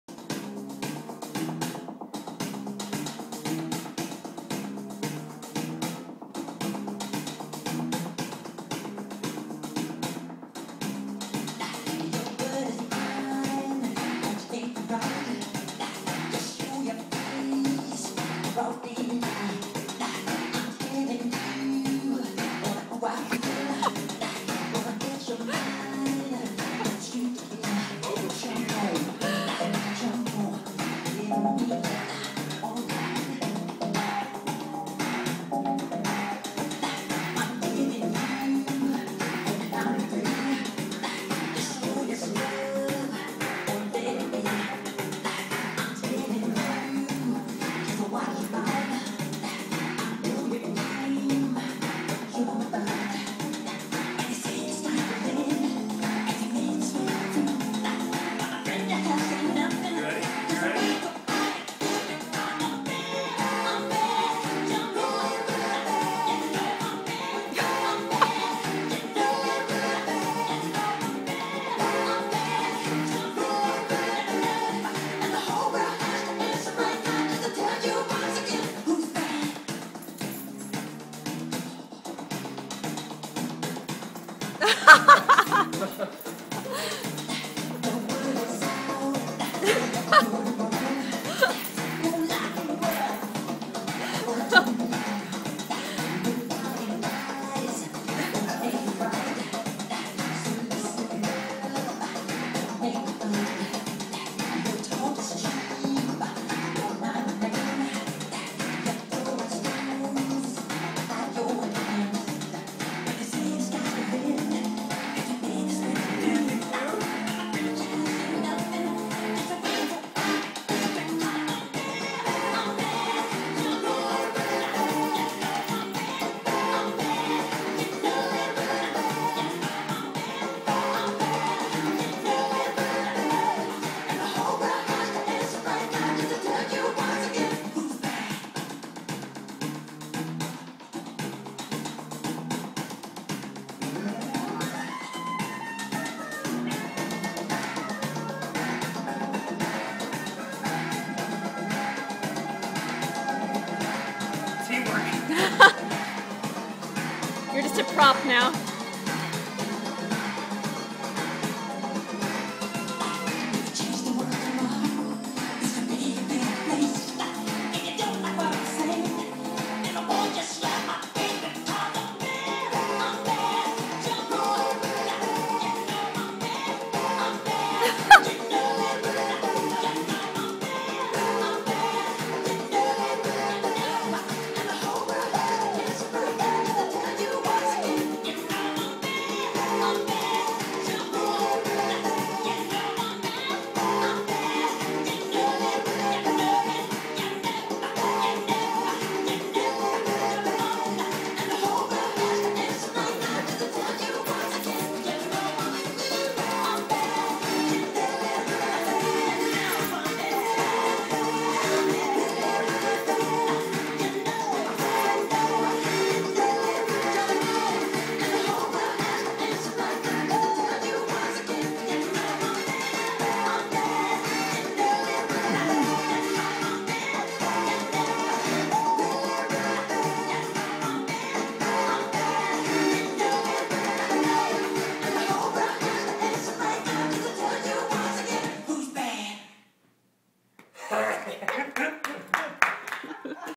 Chase's FUNNY Birthday Dance!!!
After his birthday dinner with the family, Chase asked us to put on some music because he wanted to DANCE!  He requested Michael Jackson, of course... This kid rules.